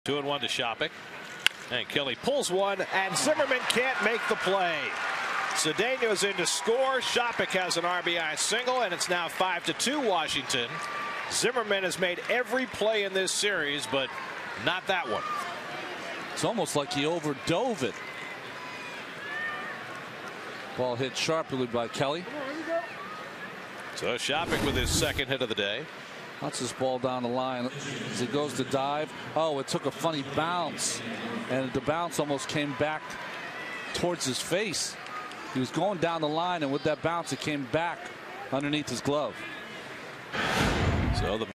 2012/08/19 Shoppach's RBI single
NYM@WSH: Shoppach deflects an RBI single in eighth